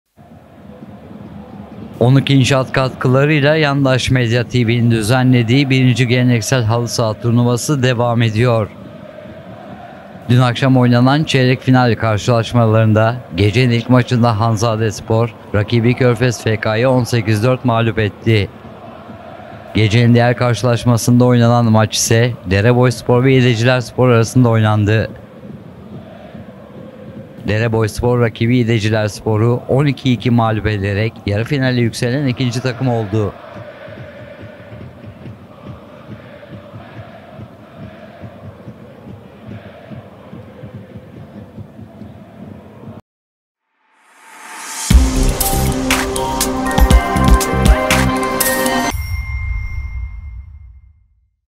💥TURNUVADA YARI FİNALE ÇIKAN İLK TAKIMLAR BELLİ OLDU.
💥Onuk İnşaat katkılarıyla Yandaş Medya Tv’nin düzenlediği 1. Geleneksel halı saha turnuvası devam ediyor. Dün akşam oynanan Çeyrek Final karşılaşmalarında gecenin ilk maçında Hanzadespor rakibi Körfez Fk’yı 18-4 mağlup etti.
Gecenin diğer karşılaşmasında oynanan maç ise Dereboyuspor ve İğdecilerspor arasında oynandı. Dereboyuspor rakibi İğdecilersporu 12-2 mağlup ederek yarı finale yükselen ikinci takım oldu. Bu sonuçlarla Yarı Finale yükselen ilk iki takım Hanzadespor ve Dereboyuspor oldu.